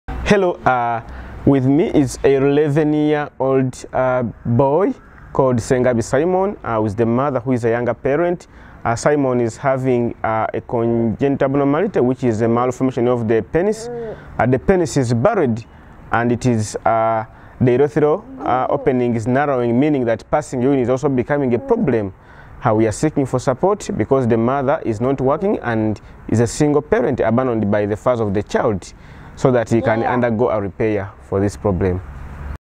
Hoping to fund baby Simon's operation
11 month old Simon has a congenital defect that means he cannot pass urine normally.
Without surgery the problem will get worse and he will be at risk of developing kidney problems.  The surgery is simple but would have a huge impact on Simon's life and will mean his mum can enjoy seeing him taking his first steps without having to worry about his condition.